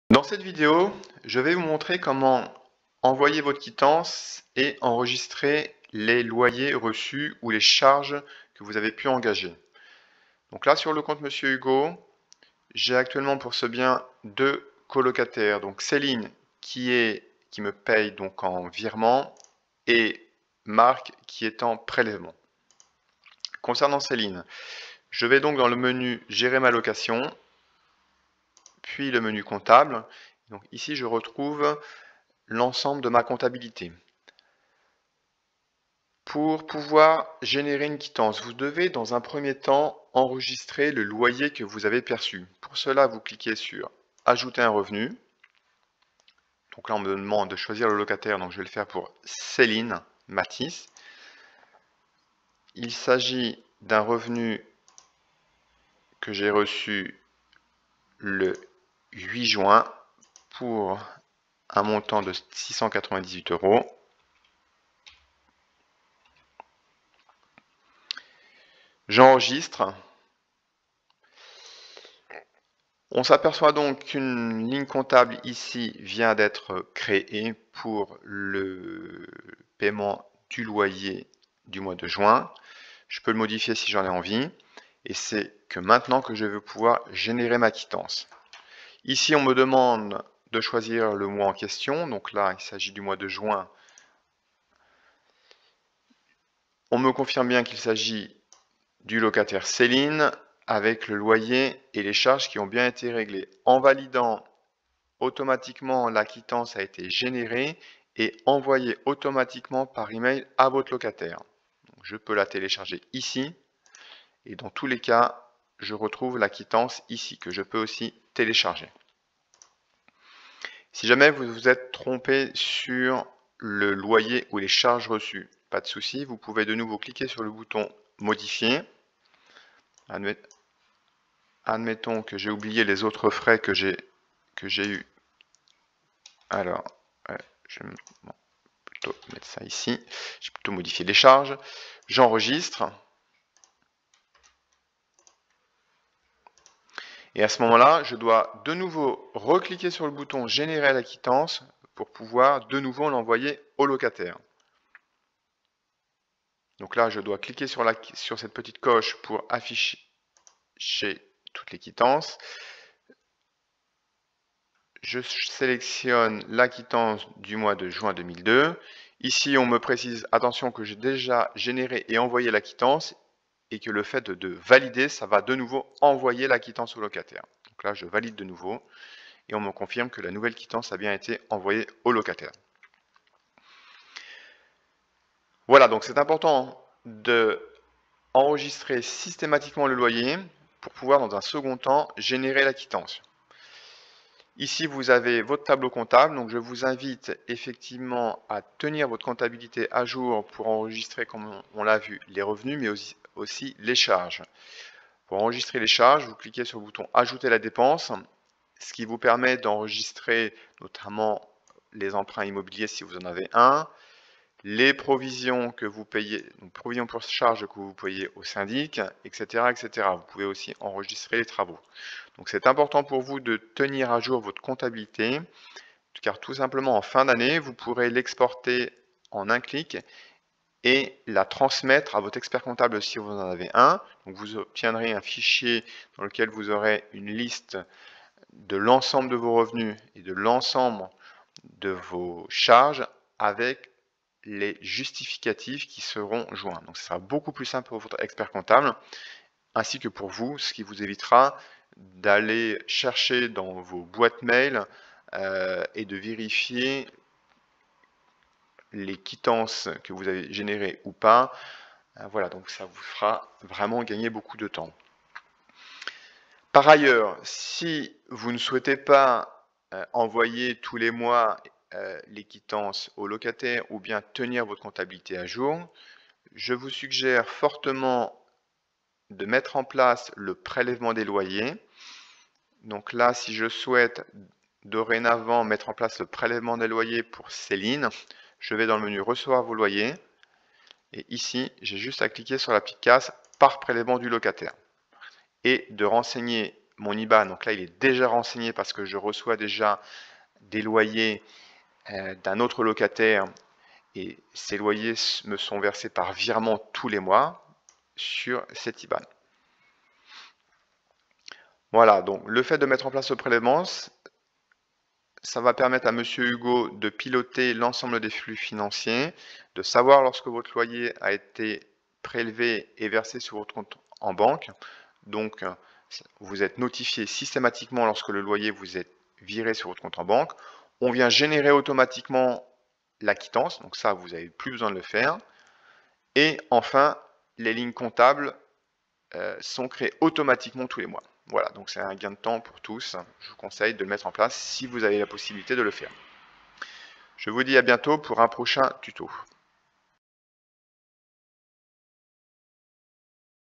Comment générer votre quittance en 1 clic ?
Dans cette vidéo vous allez apprendre à :
- Générer votre quittance via Monsieur Hugo
- Enregistrer vos revenus et charges pour tenir votre comptabilité à jour

En bonus, je vous montre comment automatiser à 95% votre gestion locative en mettant en place le prélèvement des loyers.

Monsieur Hugo : la nouvelle plateforme de gestion locative élue "L'une des 5 meilleures appli immobilières" par le magazine Challenges.